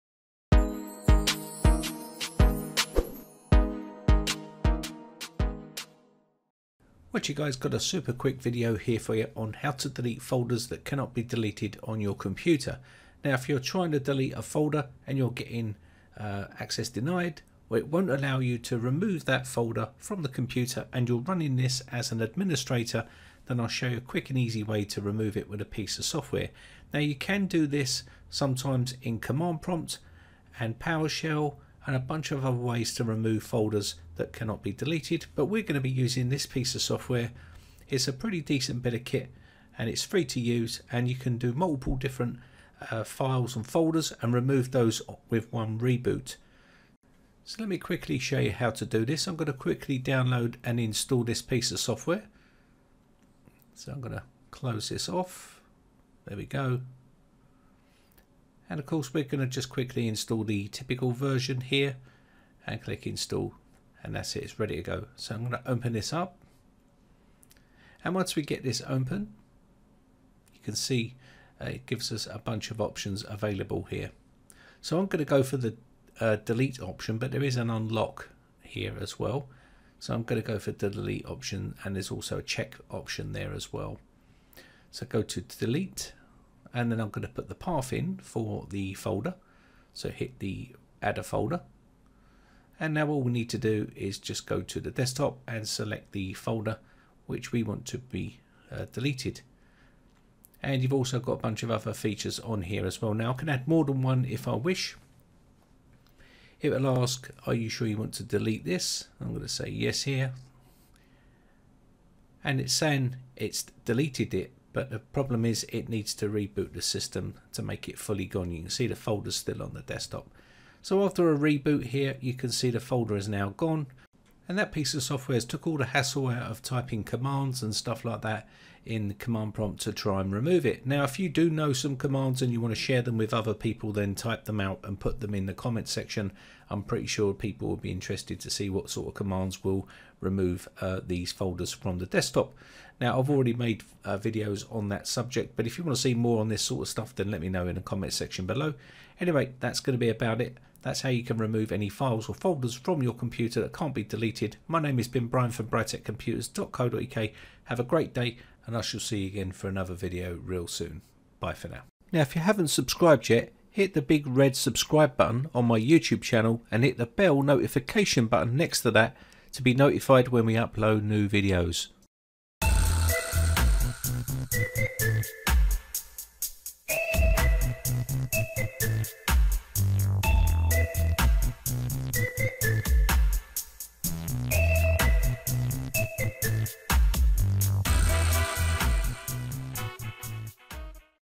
How to Delete Folder That Cannot Be Deleted
Got a file or folder that won't let you delete? getting access denied error when you try to delete it? don't worry, I will show you a simple way on How To Delete A Folder That Won't Delete. No commands need in command prompt, just a simple program that will delete file on reboot.  So even if you can't delete folder in windows 10
 and receiving access denied. I will show you step by step.